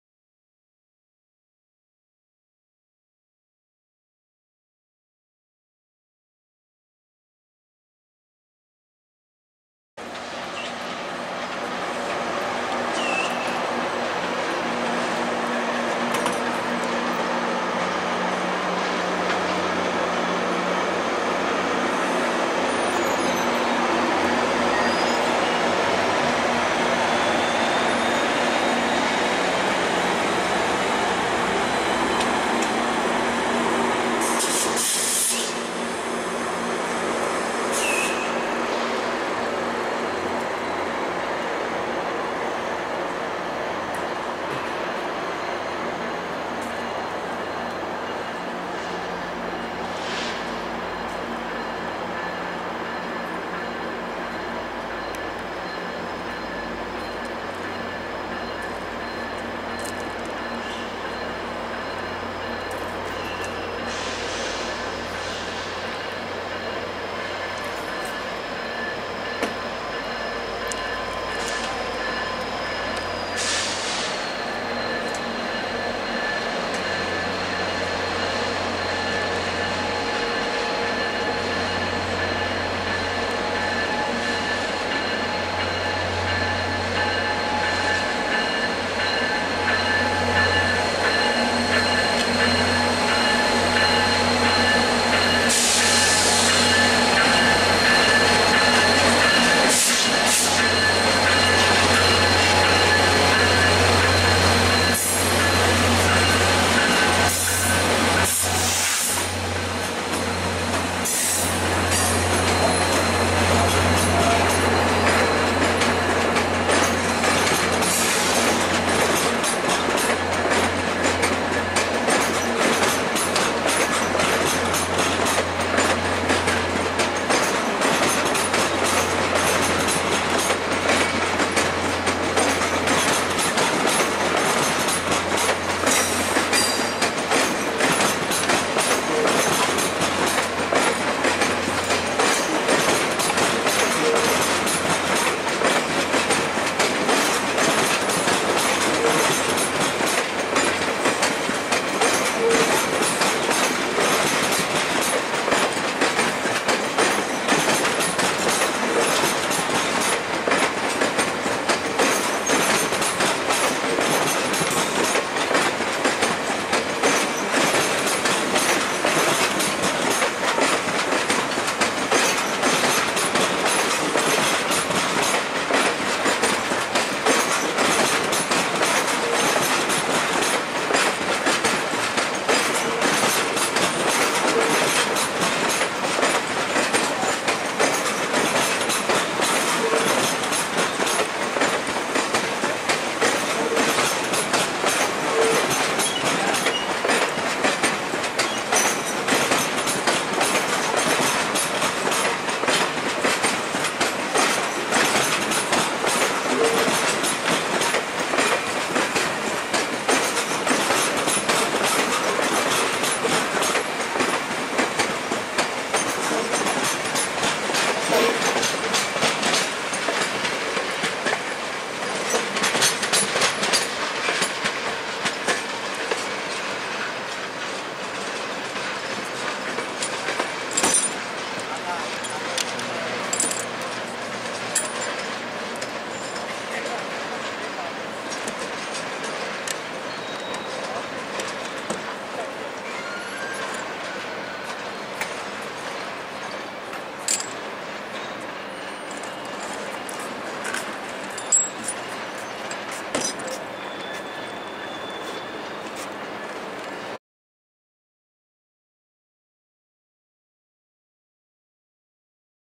The Canadian Departs Toronto's Union Station (October 5th 2022)
VIA Rail's flagship train, The Canadian, departs Toronto's Union Station bound for Vancouver, British Columbia on October 5th, 2022.

The train is still at its' full summer length and is lead by F40PH-2 units #6440 and #6441.